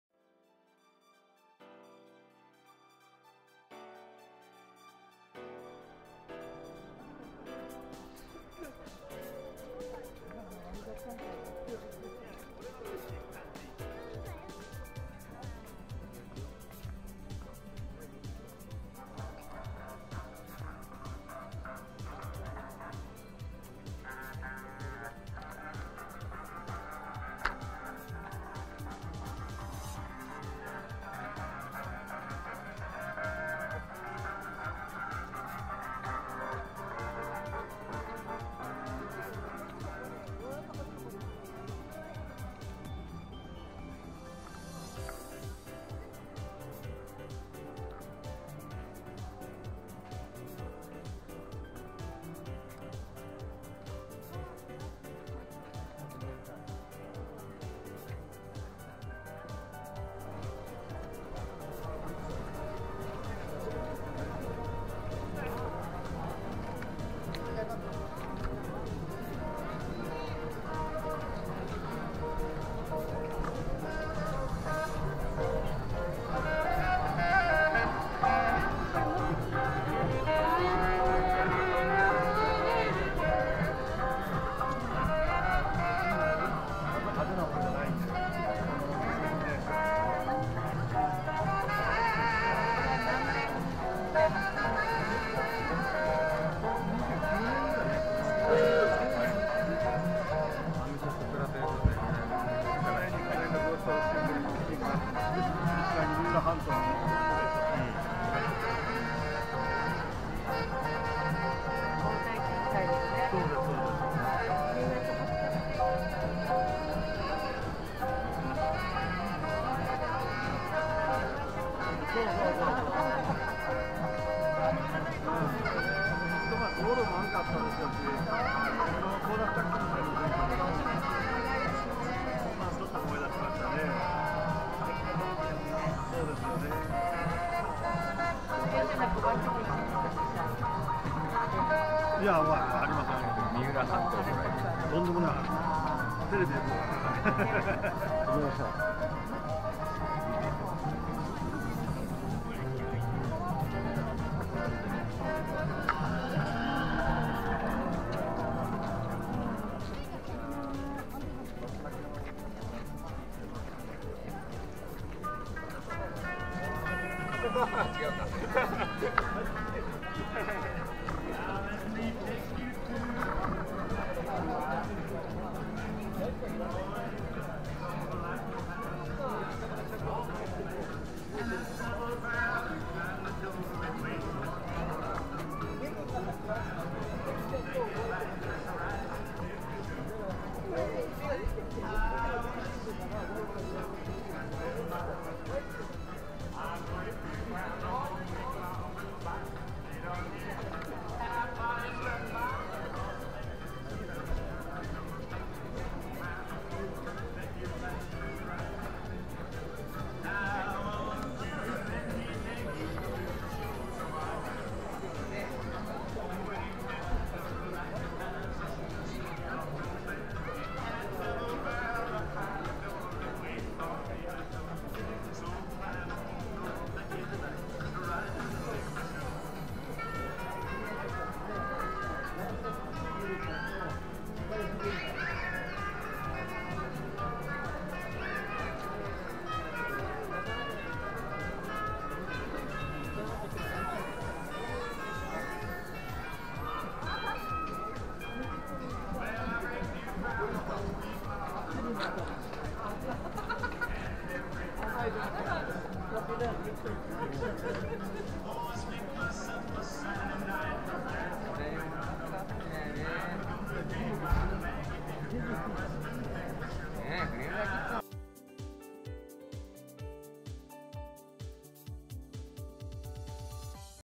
桐生散歩・ 第13回クラシックカー ・場内散歩 ①
第13回クラシックカーフェスティバルｉｎ桐生が群馬大学理工学部　桐生キャンパスで２０１８年１１月４日（日曜日）９時30分から開催されました。
 場内散歩その①　です。